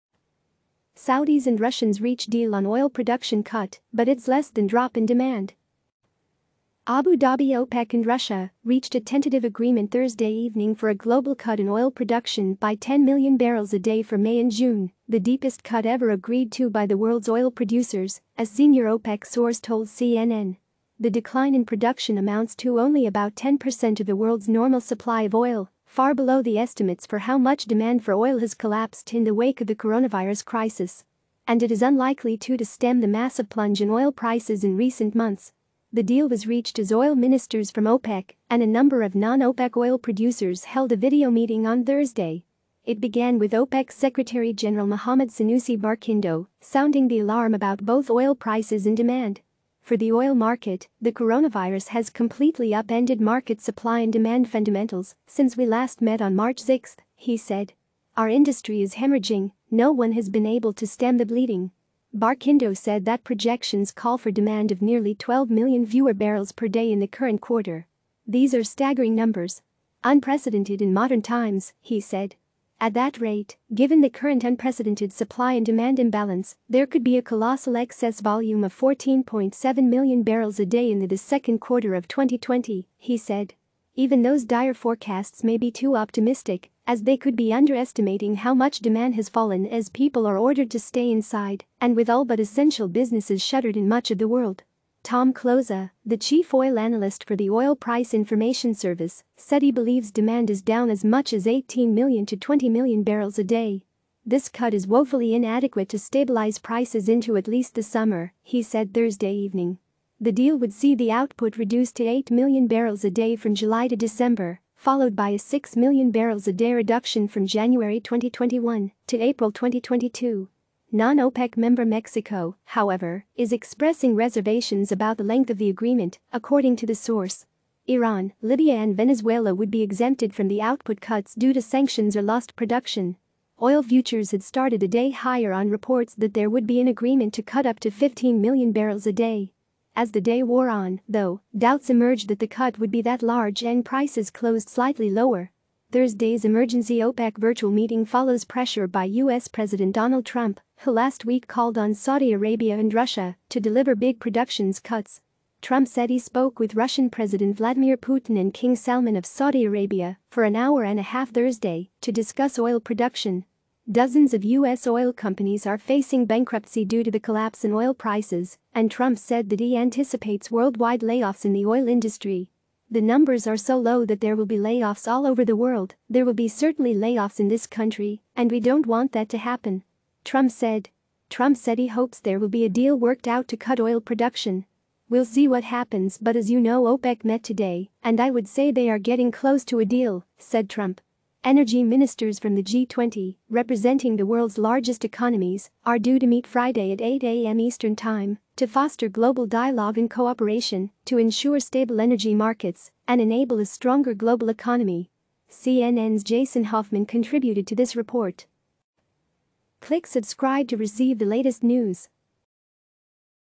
Saudis and Russians reach deal on oil production cut, but it's less than drop in demand
Abu Dabi

OPEC and Russia reached a tentative agreement Thursday evening for a global cut in oil production by 10 million barrels a day for May and June, the deepest cut ever agreed to by the world's oil producers, a senior OPEC source told CNN.

The decline in production amounts to only about 10% of the world's normal supply of oil, far below the estimates for how much demand for oil has collapsed in the wake of the coronavirus crisis. And it is unlikely to to stem the massive plunge in oil prices in recent months.

The deal was reached as oil ministers from OPEC and a number of non-OPEC oil producers held a video meeting on Thursday. It began with OPEC Secretary General Mohammad Sanusi Barkindo sounding the alarm about both oil prices and demand.

"For the oil market, [the coronavirus] has completely up-ended market supply and demand fundamentals since we last met on March 6," he said. "Our industry is hemorrhaging; no one has been able to stem the bleeding."

Barkindo said that projections call for demand of nearly 12 million fewer barrels per day in the current quarter. "These are staggering numbers! Unprecedented in modern times," he said.

At that rate, "Given the current unprecedented supply and demand imbalance there could be a colossal excess volume of 14.7 million barrels a day in the the second quarter of 2020," he said.

Even those dire forecasts may be too optimistic, as they could be underestimating how much demand has fallen as people are ordered to stay inside and with all but essential businesses shuttered in much of the world.

Tom Kloza, the chief oil analyst for the Oil Price Information Service, said he believes demand is down as much as 18 million to 20 million barrels a day. "This cut is woefully inadequate to stabilize prices into at least the summer," he said Thursday evening.

The deal would see the output reduced to 8 million barrels a day from July to December followed by a 6 million barrels a day reduction from January 2021 to April 2022. ​

Non-OPEC member Mexico, however, is expressing reservations about the length of the agreement, according to the source.

Iran, Libya and Venezuela would be exempted from the output cuts due to sanctions or lost production.

Oil futures had started the day higher on reports that there would be an agreement to cut up to 15 million barrels a day. As the day wore on, though, doubts emerged that the cut would be that large and prices closed slightly lower.

Thursday's emergency OPEC virtual meeting follows pressure by US President Donald Trump, who last week called on Saudi Arabia and Russia to deliver big productions cuts.

Trump said he spoke with Russian President Vladimir Putin and King Salman of Saudi Arabia for an hour and a half Thursday to discuss oil production.

Dozens of US oil companies are facing bankruptcy due to the collapse in oil prices and Trump said that he anticipates worldwide layoffs in the oil industry.

"The numbers are so low that there will be layoffs all over the world, there will be certainly layoffs in this country and we don't want that to happen.," Trump said.

Trump said he hopes there will be a deal worked out to cut oil production. "We'll see what happens but as you know OPEC met today and I would say they are getting close to a deal," said Trump.

Energy ministers from the G20, representing the world's largest economies, are due to meet Friday at 8 am ET "to foster global dialogue and cooperation to ensure stable energy markets and enable a stronger global economy."

-- CNN's Jason Hoffman contributed to this report